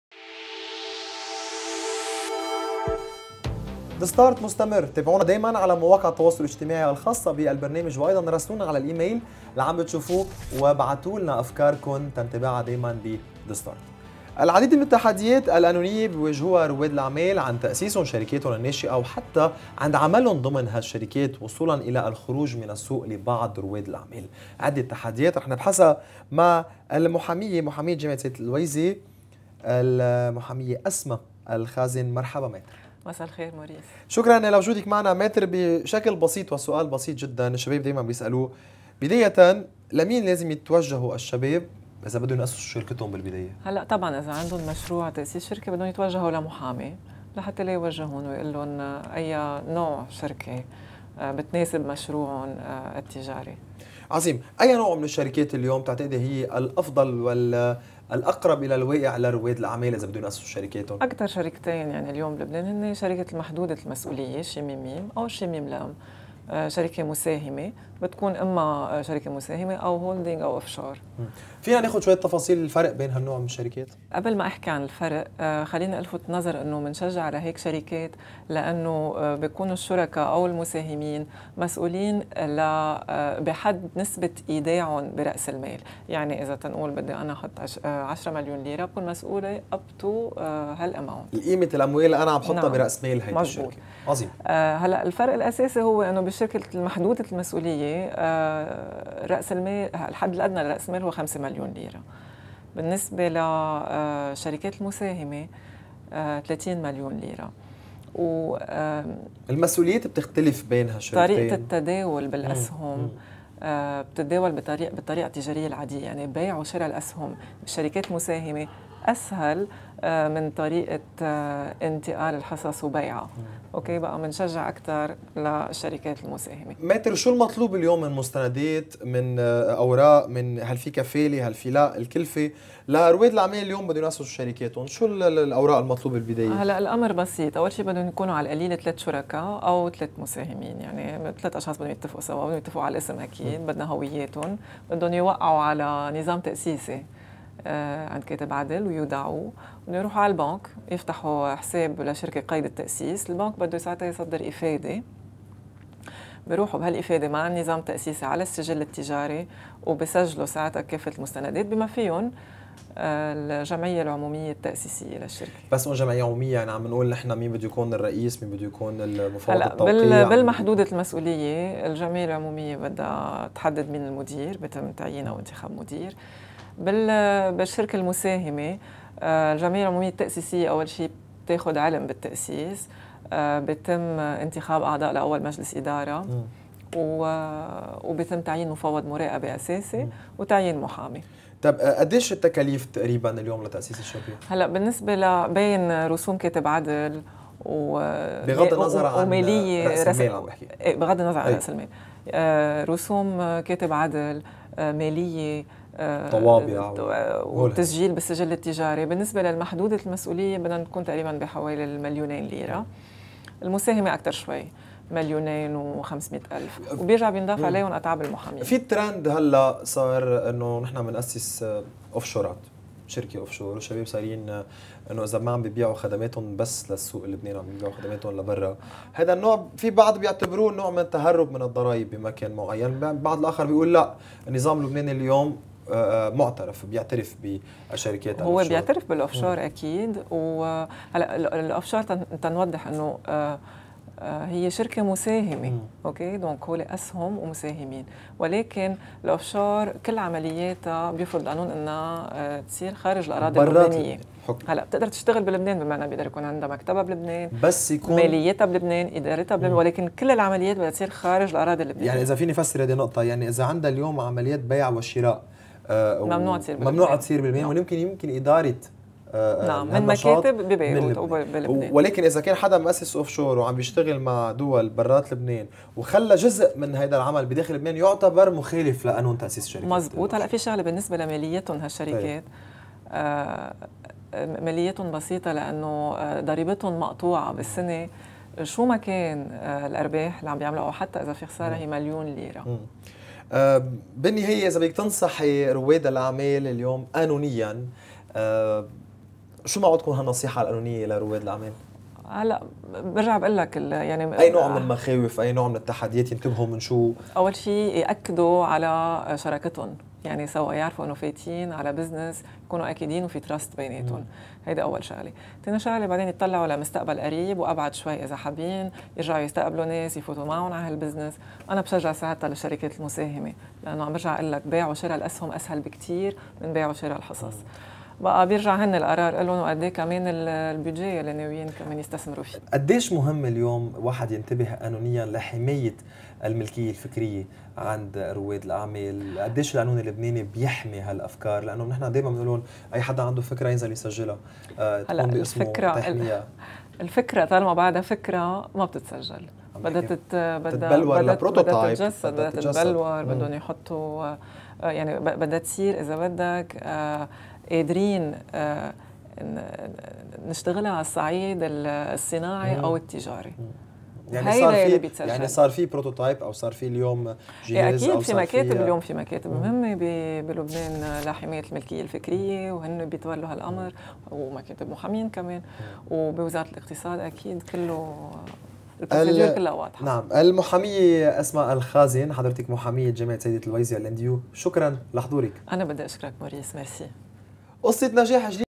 TheStart
Attorney Asma El Khazen, University legal Counsel discussing the legal aspects of owning a company.